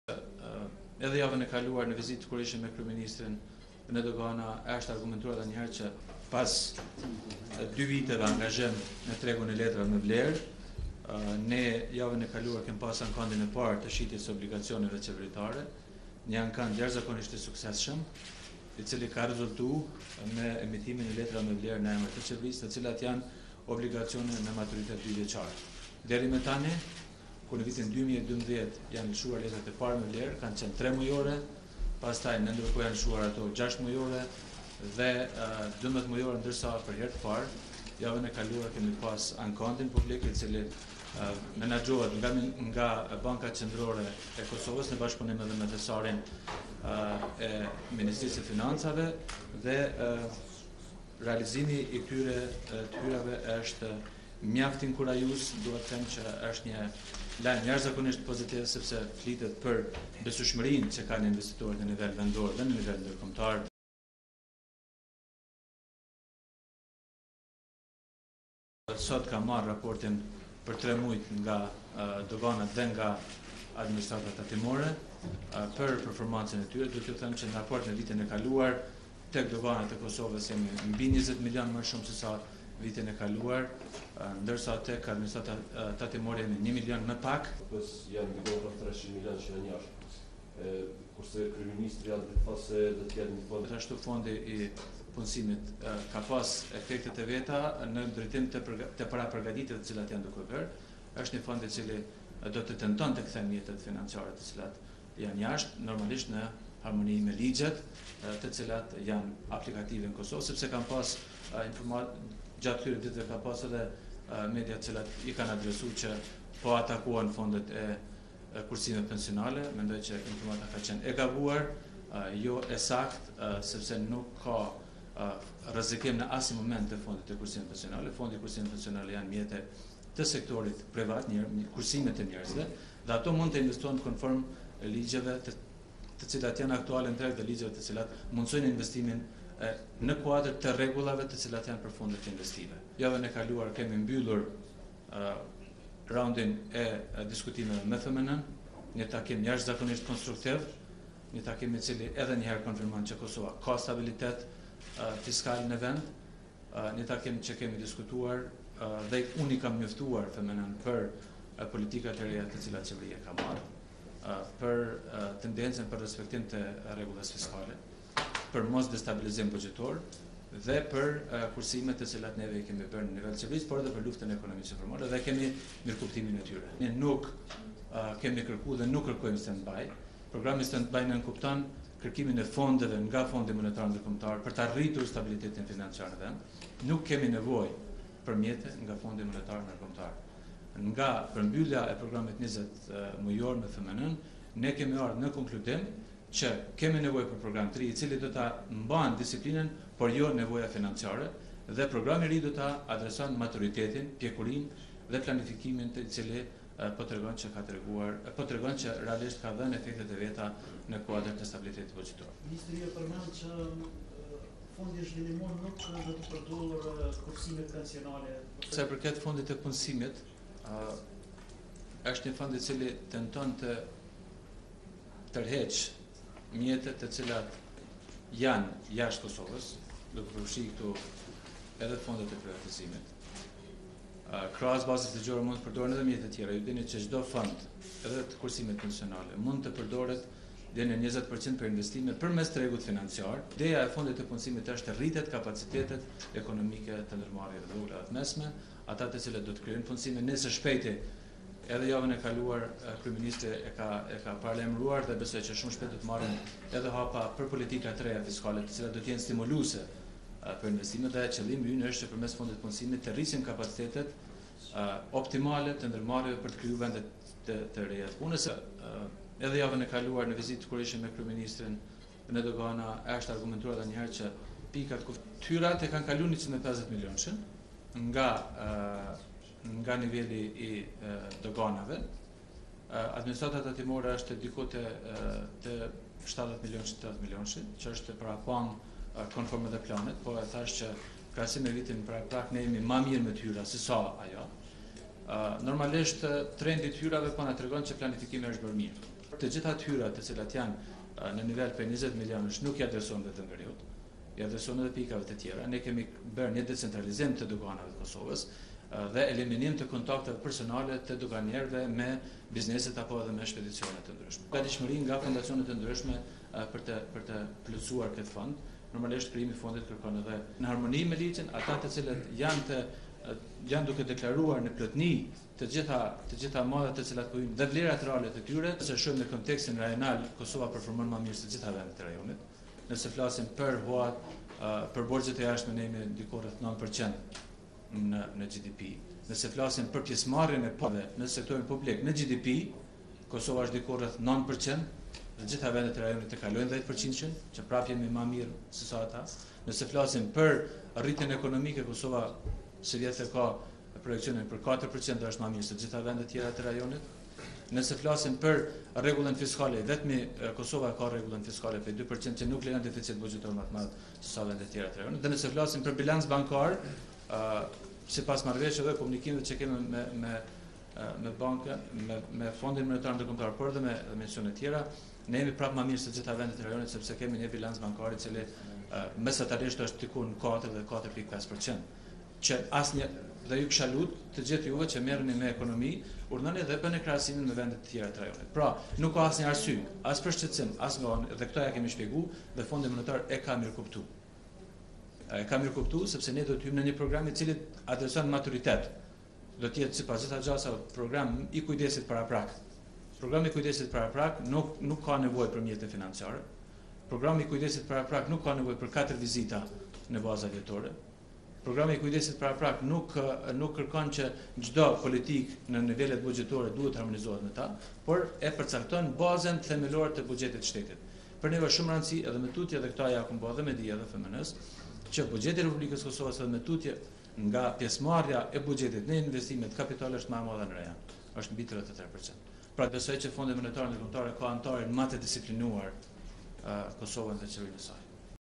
Obligacionet qeveritare me maturitet 2 vjeçar
Obligacionet qeveritare me maturitet 2 vjeçar shprehin besimin e investitorëve dhe stabilitetin financiar të Kosovës

Prishtinë -- 02 prill 2014
Interesimi i jashtëzakonshëm për blerjen e obligacioneve qeveritare me maturitet dy vjeçar është një lajm jashtëzakonisht pozitiv sepse shpreh besueshmërinë që kanë investitorët në nivel vendor por edhe ndërkombëtar për blerjen e letrave me vlerë, të cilat tregojnë stabilitet financiar, ka vlerësuar sot ministri i Financave Besim Beqaj.

Në konferencën për media ministri Beqaj tha se pas dy viteve angazhim në tregun e letrave me vlerë, javën e kaluar është realizuar ankandin i parë i shitjes së obligacioneve qeveritare, emetimi i letrave me vlerë në emër të Qeverisë, të cilat kanë maturitet 2 vjeçar.